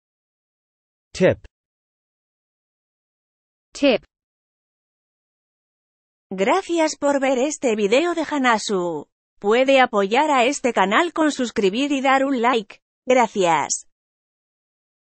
How to pronounce 'Tip' (British English)
How to pronounce 'Tip' in British English.
Comment prononcer 'Tip' en anglais britannique.
Como pronunciar 'Tip' en inglés británico.
Wie man 'Tip' mit britischem Akzent ausspricht.
「Tip」の英語の発音の仕方